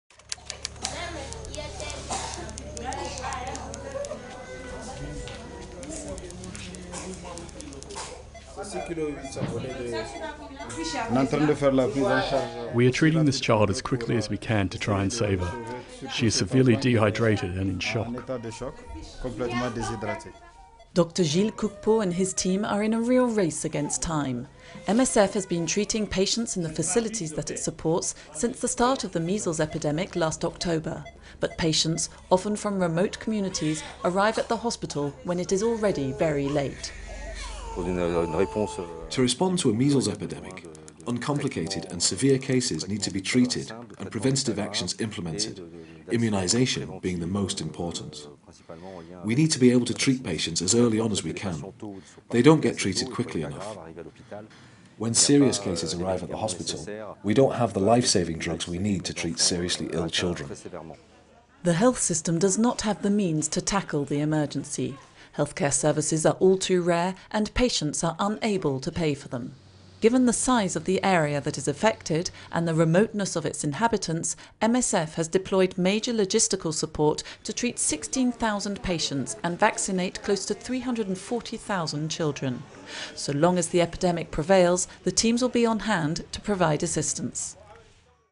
Battling a Measles Epidemic in Northeast Congo
Following a recent malaria epidemic in the Democratic Republic of Congo, a measles outbreak has spread through some 20 health zones in the the country's Orientale Province. Doctors Without Borders/Médecins Sans Frontières (MSF) has deployed major logistical support to treat 16,000 patients and vaccinate close to 340,000 children in the affected regions.